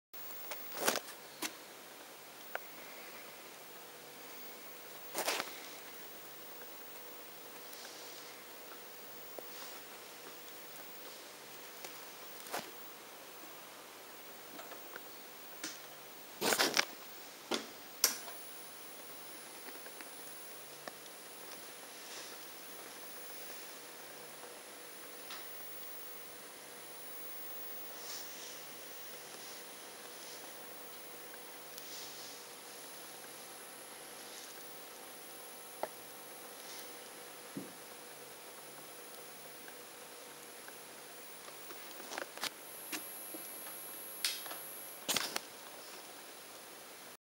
Sammelröhre nach Crookes mit fluoreszierendem Mineralstein Amethyst
Funktion einer Sammelröhre nach Crookes mit einem fluoreszierendem Mineralstein Amethyst